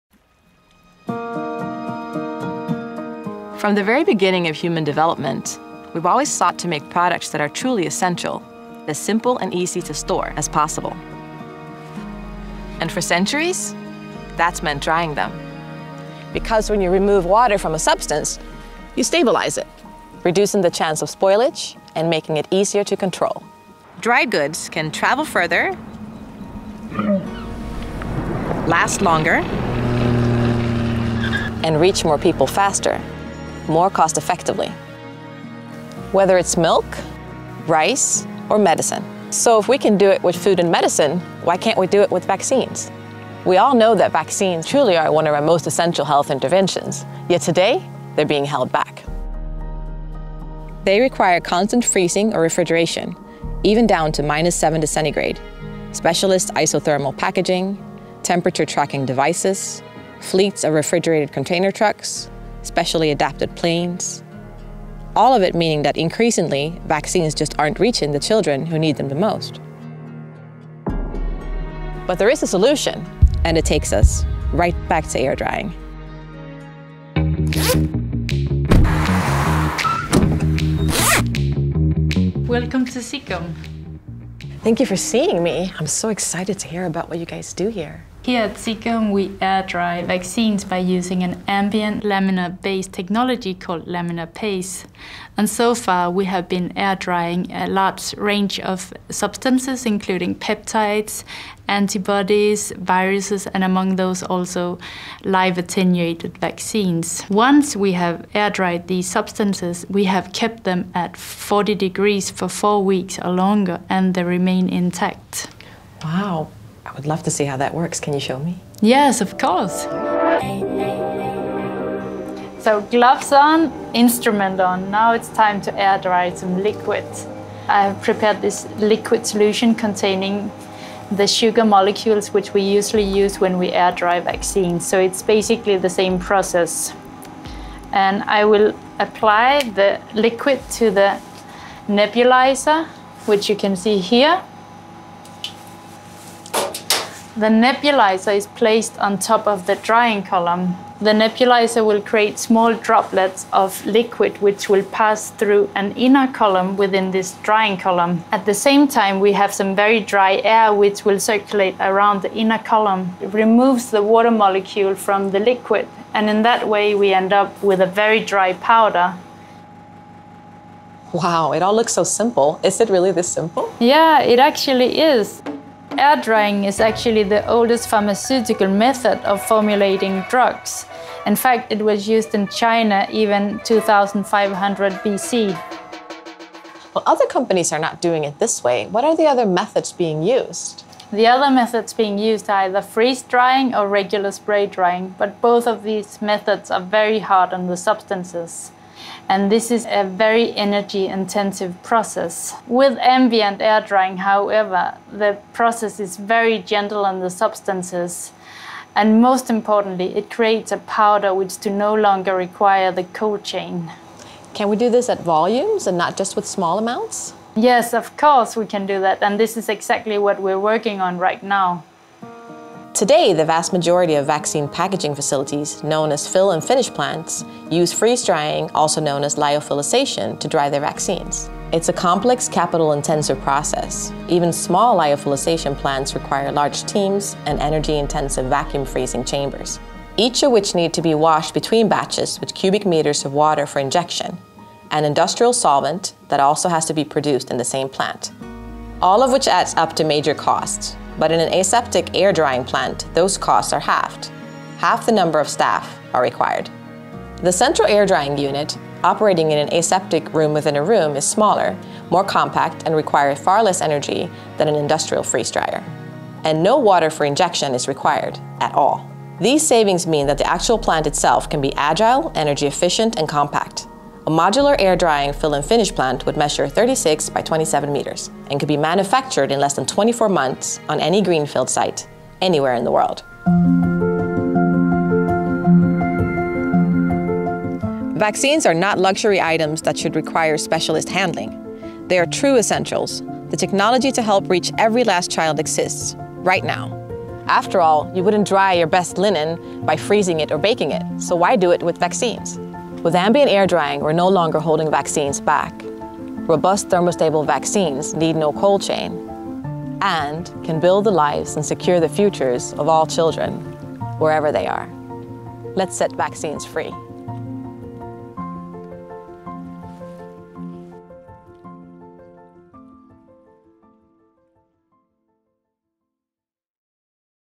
Set Vaccines Free: How ambient air-drying can create thermostable vaccines that need no Cold Chain.
Dry powder vaccines could be stored and transported anywhere in the  world with no need for refrigeration or freezing. How does Ziccum produce them? What are the savings and benefits? How does it plan to scale production up?